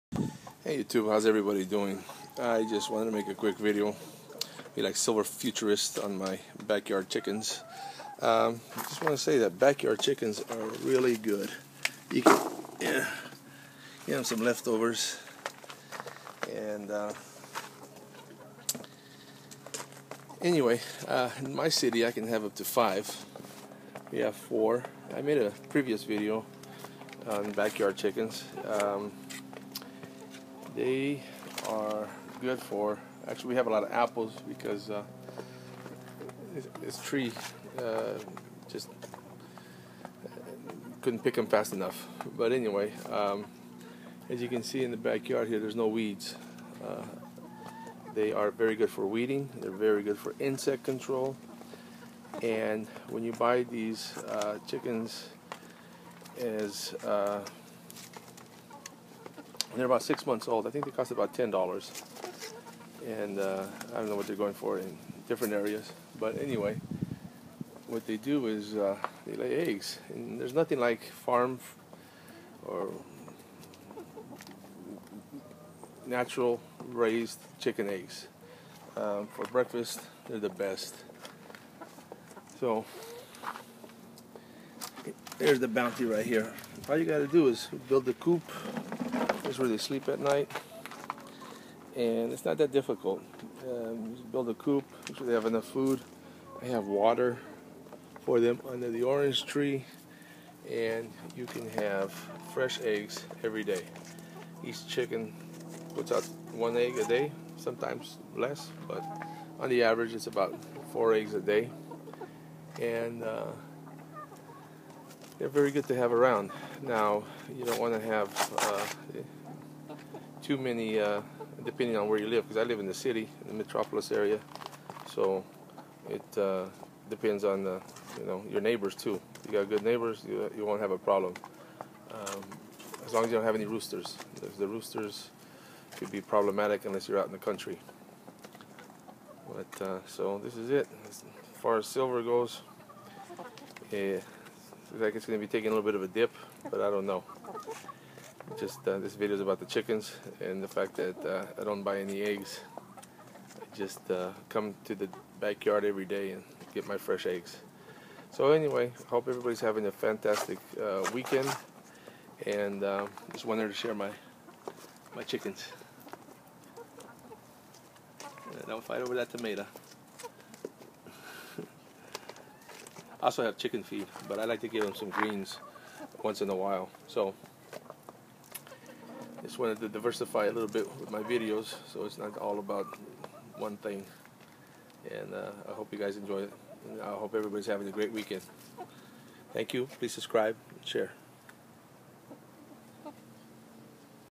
HOW TO KEEP BACKYARD CHICKENS
Backyard Chickens are easy to keep they eat almost anything
Feed 50lb bag cost about $15.00 can last up to 4 months if you also feed them leftovers.
You can train you chickens to lay there eggs in a spot buy using a golf or ping pong ball
They need to have cover in the laying area.